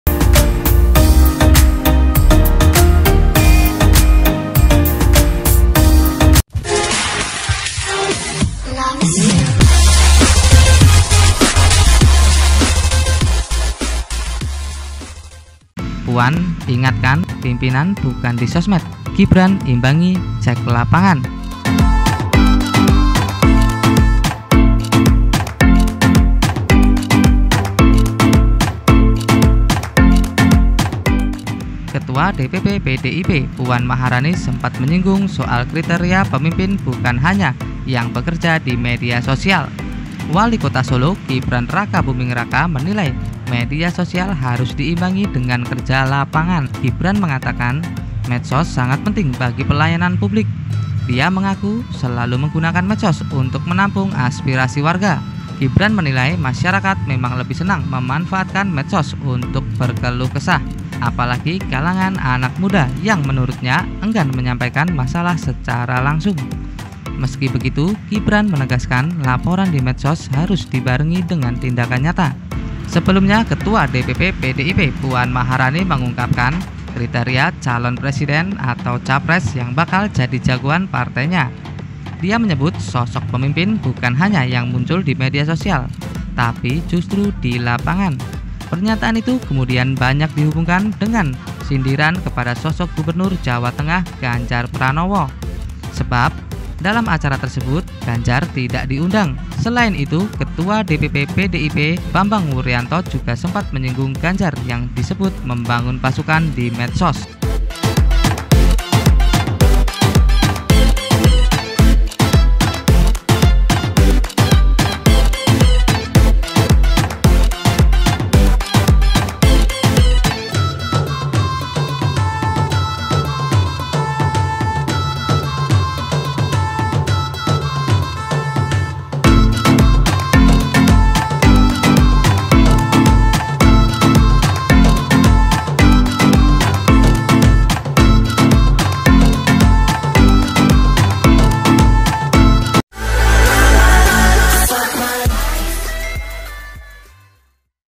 Puan Ingatkan Ganjar Jangan Hanya GANDANG di Medsos !!! ... Gibran Ikut Berkomentar ...
Puan Ingatkan Pemimpin Bukan di Sosmed, Gibran Imbangi Cek Lapangan

Gibran mengatakan medsos sangat penting bagi pelayan publik. Dia mengaku selalu menggunakan medsos untuk menampung aspirasi warga.

"Penting sekali. Untuk pengaduan publik, keluhan-keluhan warga kan lebih efektif memanfaatkan medsos. Yang sepele aja, aspal bolong, drainase mampet," kata Gibran di sela-sela kegiatannya di Solo, Kamis (27/5/2021).

Gibran menilai masyarakat memang lebih senang memanfaatkan medsos untuk berkeluh kesah. Apalagi kalangan anak muda, menurutnya enggan menyampaikan masalah secara langsung.

"Warga kan lebih nyaman lewat WA, Instagram, DM Facebook. Apalagi anak muda, kalau disuruh ke balai kota mengeluh kan nggak mau, kan lebih efektif lewat medsos," ujar dia.

Meski begitu, Gibran menegaskan laporan di medsos harus dibarengi dengan tindakan nyata. Dia mengaku turun langsung untuk mengecek fakta di lapangan.

"Tapi sebagai pemimpin juga harus mengecek langsung ke lapangan. Ya harus berimbang," ujarnya.

Sebelumnya, Ketua DPP PDIP Puan Maharani mengungkap kriteria calon presiden (capres) yang bakal jadi jagoan partainya.

"Pemimpin menurut saya, itu adalah pemimpin yang memang ada di lapangan dan bukan di sosmed," kata Puan usai membuka Pameran Foto Esai Marhaen dan Foto Bangunan Cagar Budaya di kantor DPD PDIP Jawa Tengah, Panti Marhen, Semarang, Sabtu (22/5).

"Pemimpin yang dilihat sama teman-temannya, sama orang-orangnya yang mendukungnya ada di lapangan," sambungnya.

Pernyataan itu kemudian banyak dihubungkan dengan sindiran kepada sosok Gubernur Jawa Tengah Ganjar Pranowo. Sebab dalam acara tersebut Ganjar tidak diundang. Selain itu, Ketua DPP PDIP Bambang Wuryanto juga sempat menyinggung Ganjar yang disebut membangun pasukan di medsos.

"Di dalam persepsi kita semua, Ganjar racing jadi presiden. Dia orang PDIP yang racing jadi calon presiden dengan membangun pasukannya di medsos. Kemudian dari pasukan itu kemudian nanti akan keluar elektabilitas. Dia berharap kalau elektabilitasnya tinggi, dia nanti akan mendapat rekom menjadi capres. Itu konstruksi dasarnya kan begitu," kata Bambang beberapa waktu lalu.